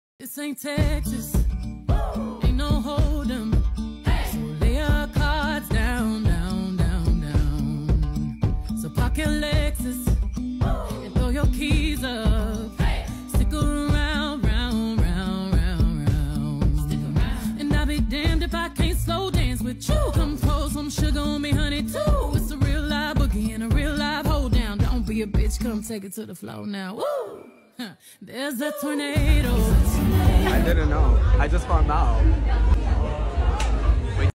Beyoncé Surprises Fashion Designer in Brooklyn
Beyoncé andTina Knowles showed up in Brooklyn at the Luar show to support Solange's son, Julez.